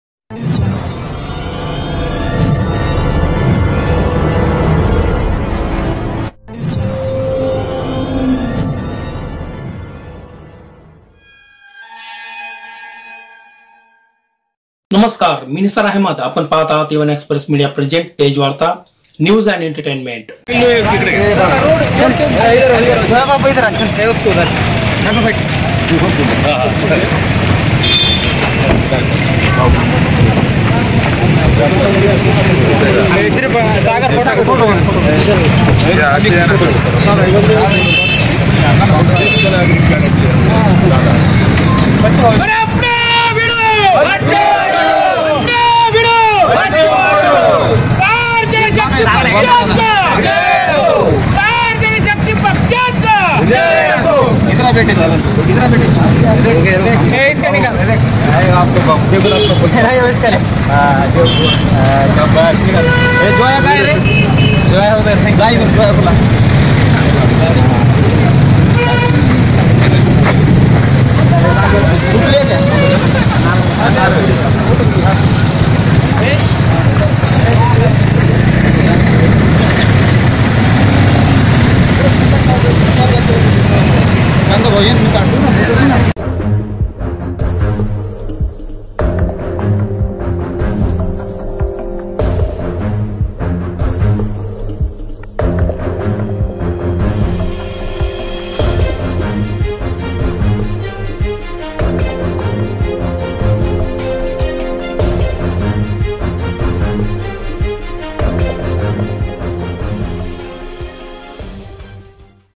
आष्टी -आ. बच्चु कडु यांचा कडयात कार्यकर्त्यांनी केला सत्कार.
तेजवार्ता न्युज